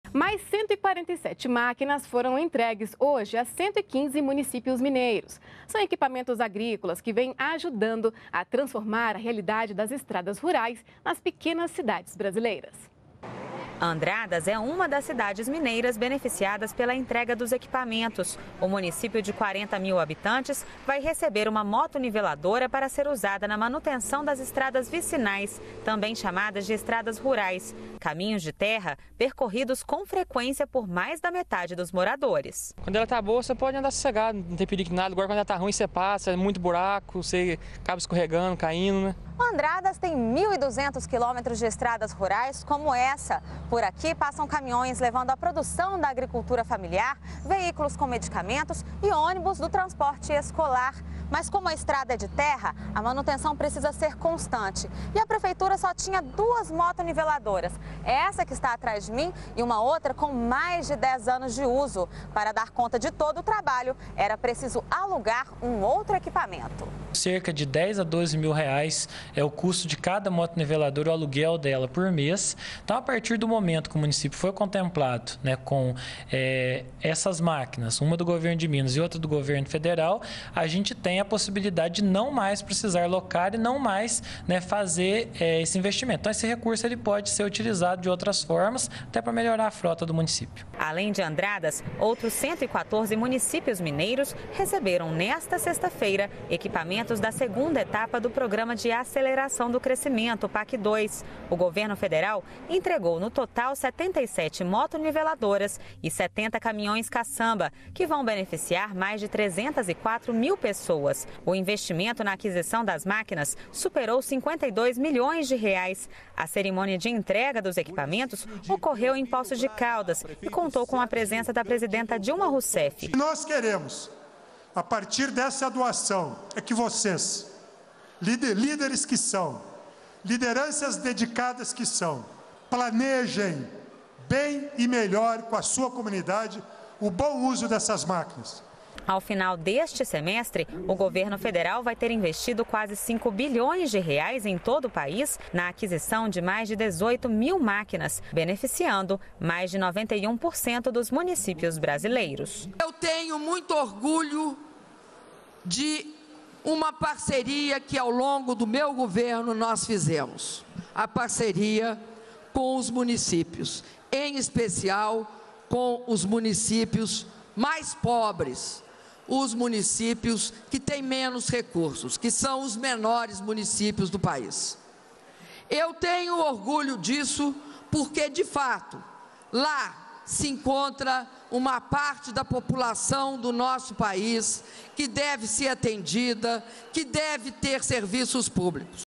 Máquinas agrícolas vão beneficiar a população de 115 cidades mineiras
NBR NOTÍCIAS -- 30.05.14: Nesta sexta-feira (30), a presidenta Dilma Rousseff participou da cerimônia de entrega de equipamentos agrícolas para cidades mineiras. O município de Andradas foi um dos 115 contemplados com 70 caminhões-caçamba e 77 motoniveladoras da segunda fase do Programa de Aceleração do Crescimento (PAC 2). O maquinário vai auxiliar nas obras de infraestrutura das áreas rurais, na recuperação e manutenção de estradas vicinais - que ligam o campo à cidade, e no escoamento da produção agrícola do estado. O investimento na compra das máquinas superou os R$ 52,2 milhões , mais de 304 mil pessoas serão beneficiadas. Com essa entrega, Minas Gerais já recebeu 2.468 máquinas distribuídas entre os municípios com menos de 50 mil habitantes.